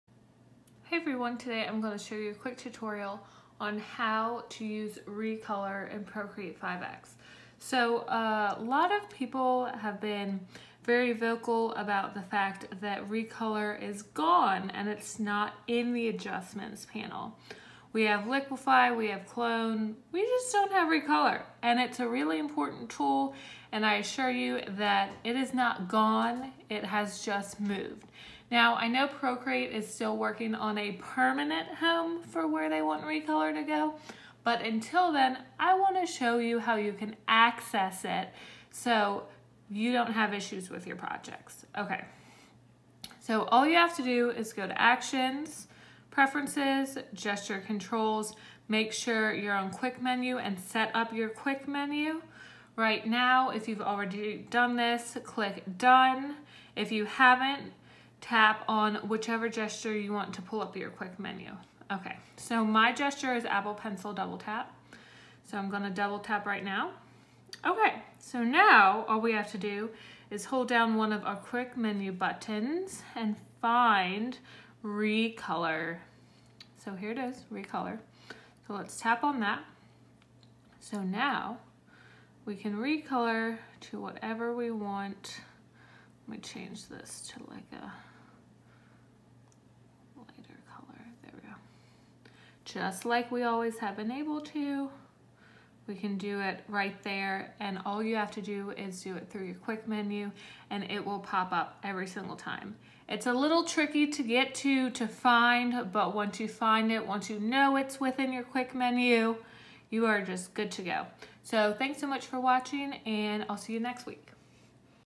How To Use The Recolor Tool In Procreate
With Procreate 5X came the release of the new Color Fill tool which is the successor to the very popular Recolor Tool. While the Color Fill tool offers some interesting new features it doesn't completely replace the Recolor tool.

While at first glance it might seem like Procreate completely took away the Recolor Tool because it's not in the Adjustments Panel, rest assured it is still on the app and you can still access it!

SHOP MY DIGITAL PRODUCTS:

WHAT I'M USING:

12.9 iPad Pro (3rd Gen)
Apple Pencil (2nd Gen)